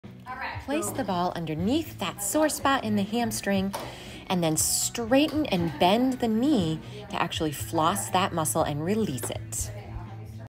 Loosen Up The Hamstrings
Hamstrings often need some blood flow to get them to loosen up.
This muscle flossing technique can be done with a tennis ball or lacrosse ball.
Perform until the spot feels less tender.